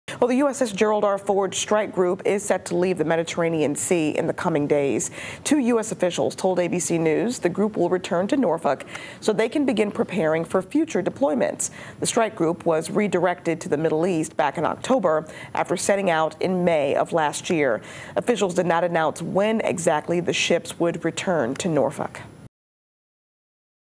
USS Gerald R. Ford aircraft carrier coming home after extended deployment
The Norfolk-based USS Gerald R. Ford aircraft carrier strike group is heading home after months of extra duty at sea providing protection for Israel.